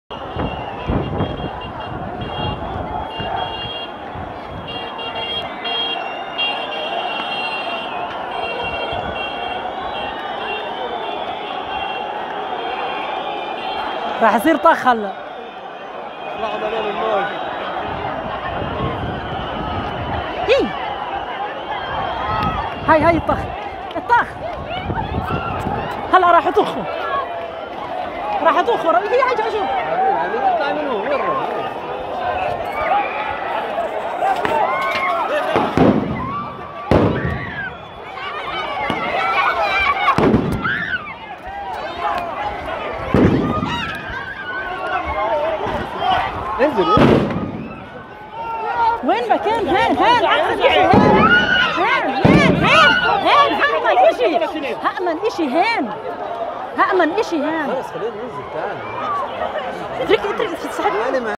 Breaking News: Israel occupation troops attack al-Aqsa again break peace agreement
Fake Israel democracy can't survive without terrorising non-Jews in the same way that ISIS did. This is at the heart of Zionist racist policy.